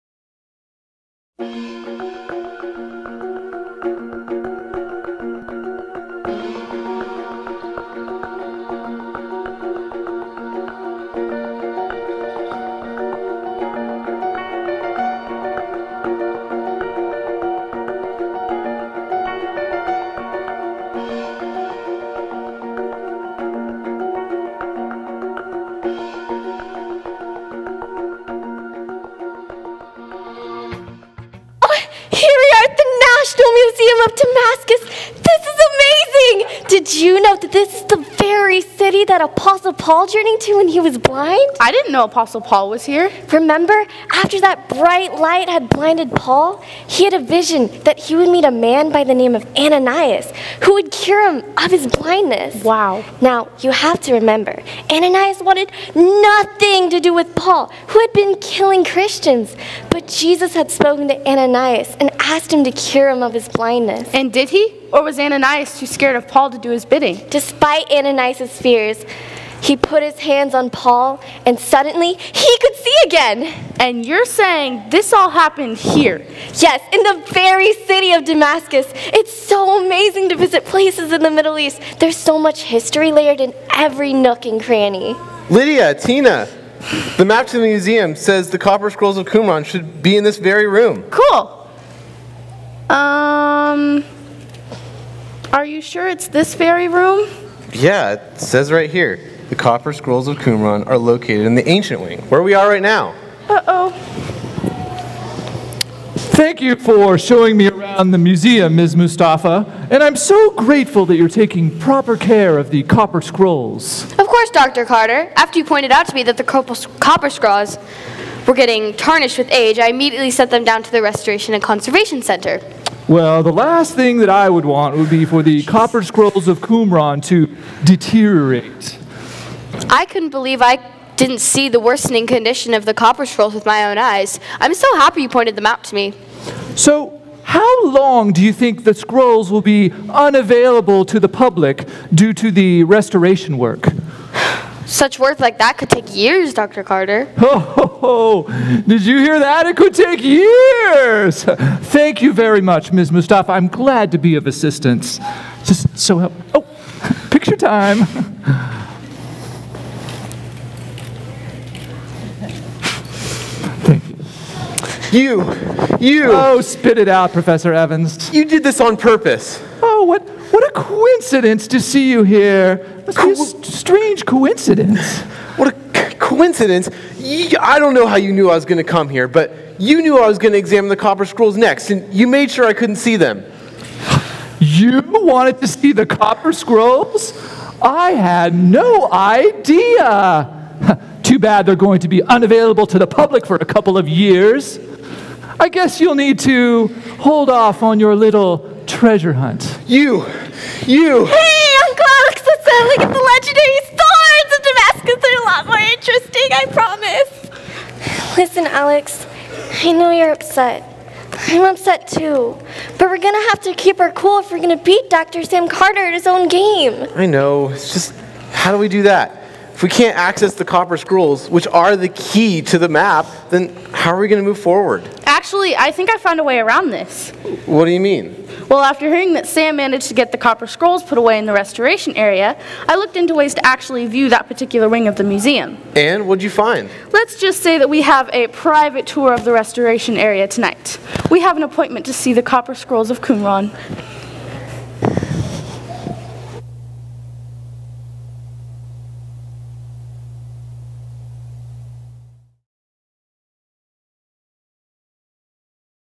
The Lost Treasure of Jerusalem. Scene Four.
Alex, Tina and Lydia arrive at the National Museum of Damascus in order to see the Copper Scrolls of Qumran. Unfortunately, Dr. Sam Carter has arrived before them and has managed to get the Copper Scrolls sent away to the restoration area. Breakaway/Vacation Bible School Play. Filmed on 7/18/2017 at University Covenant Church in Davis, CA.